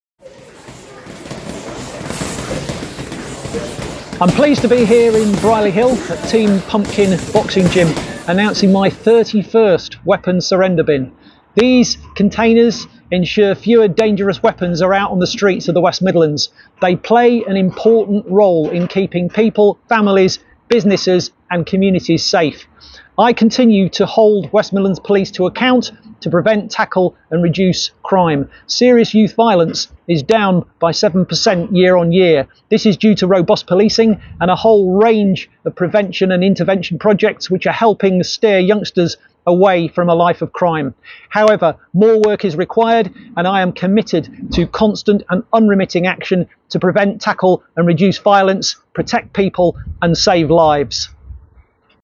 PCC announces 31st weapon surrender bin in Brierley Hill
A thriving boxing club in the heart of the Black Country has thanked West Midlands Police and Crime Commissioner, Simon Foster, for the recent installation of another weapon surrender bin.

The latest container has been positioned outside Team Pumpkin Amateur Boxing Club in Brierley Hill, taking the total of weapon bins to 31 across the region. The new bin follows four that were welcomed back in January in Coventry, Solihull, Tipton and Wolverhampton.